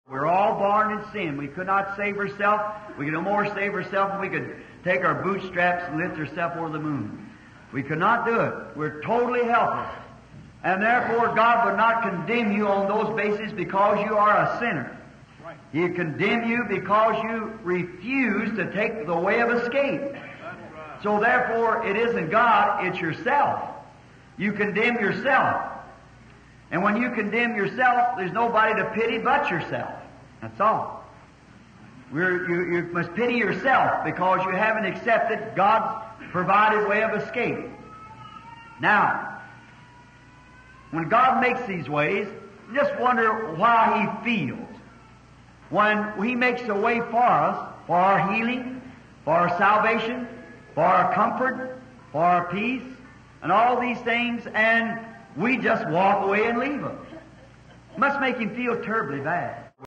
"Why?" Rv. William Branham
61-0125

Why?

We're all born in sin. We could not save ourself. We could no more save ourself, than we could take our boot straps and lift ourself over the moon. We could not do it. We're totally helpless. And therefore, God would not condemn you on those basis because you are a sinner. He'd condemn you because you refuse to take the way of escape. So therefore, it isn't God; it's yourself. You condemn yourself. And when you condemn yourself, there's nobody to pity but yourself. That's all. We're…You—you must pity yourself, because you haven't accepted God's provided way of escape.
Now, when God makes His ways, just wonder wh—how He feels when He makes a way for us, for our healing, for our salvation, for our comfort, for our peace, and all these things, and we just walk away and leave them. It must make Him feel terribly bad.

📖 Daily Bread

Ephesians 2:8

For by grace are ye saved through faith; and that not of yourselves: it is the gift of God: